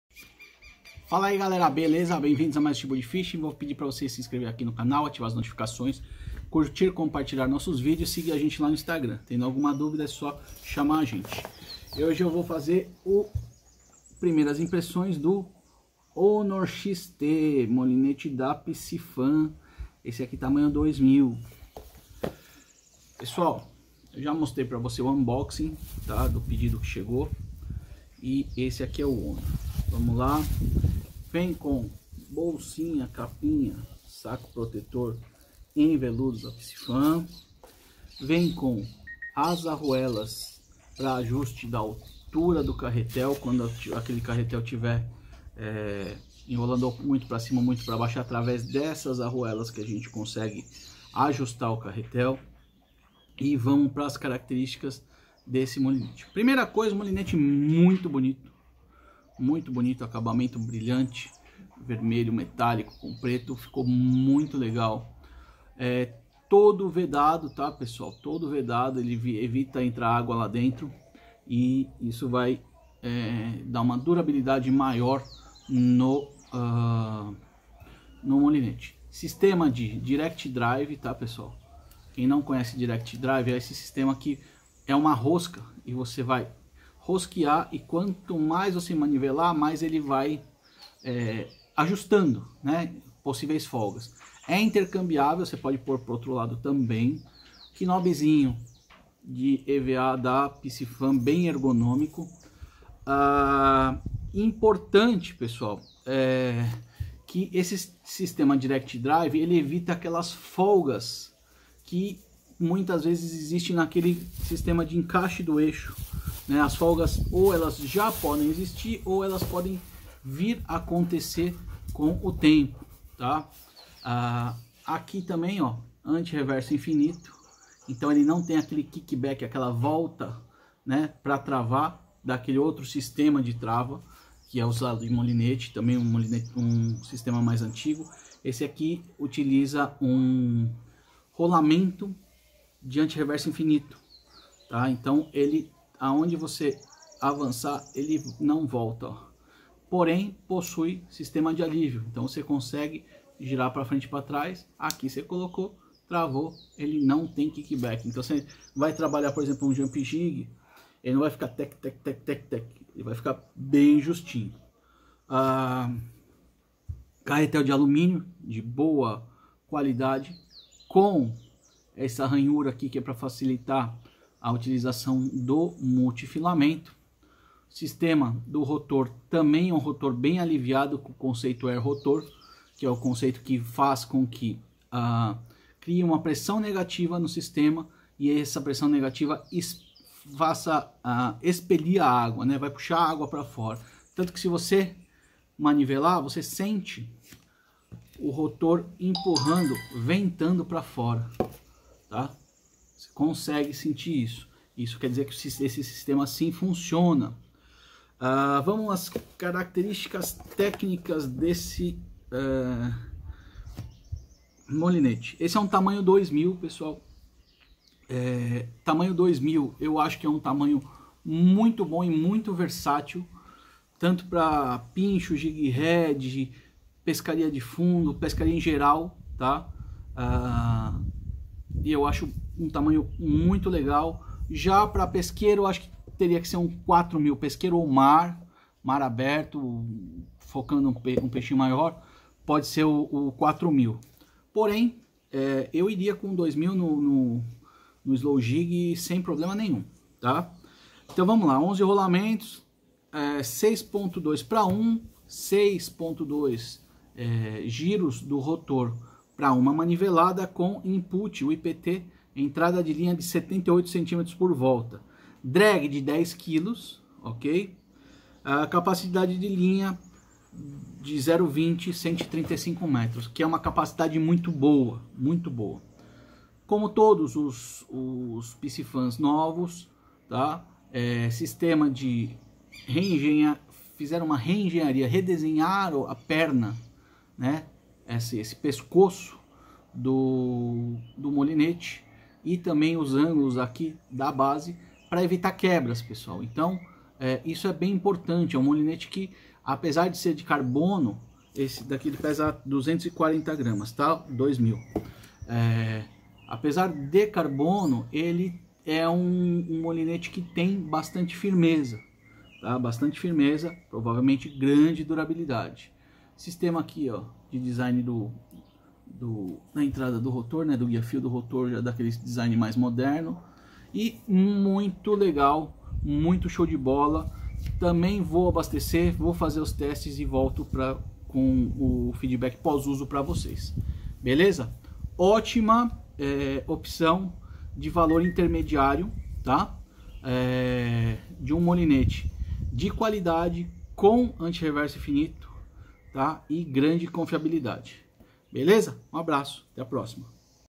Molinete Honor XT 2000 Piscifun
Neste video eu mostro e falo sobre as minhas primeiras impressões sobre o Molinete Honor XT 2000 da Piscifun